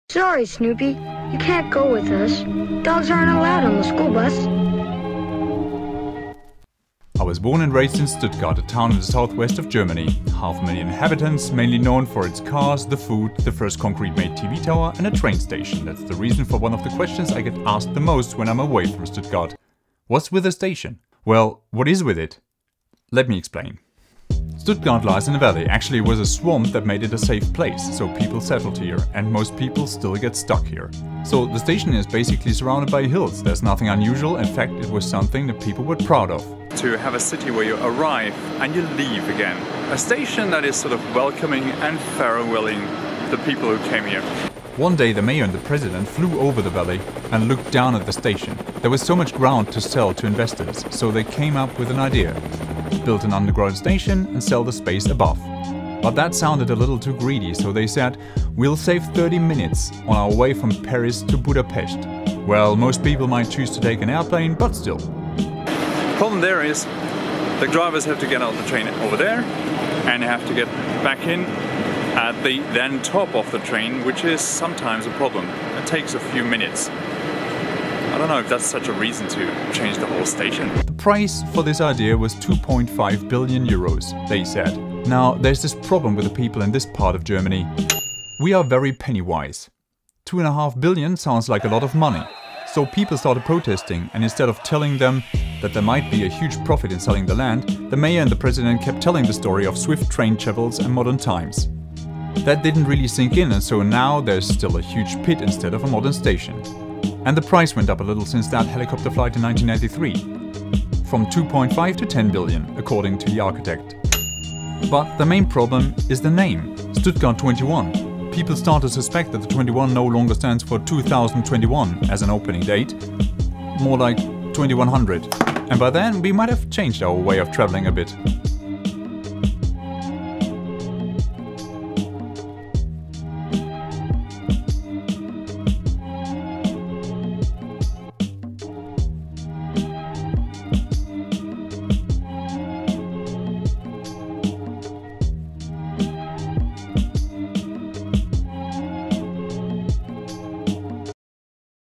What is Stuttgart 21 ?
To all those who don't really understand the term "Stuttgart 21"

The "Bahnprojekt Stuttgart-Ulm" is one of the most ambitious german projects (after the Berlin Airport and global peace.
But what is it? And why are they tearing the whole city up for it?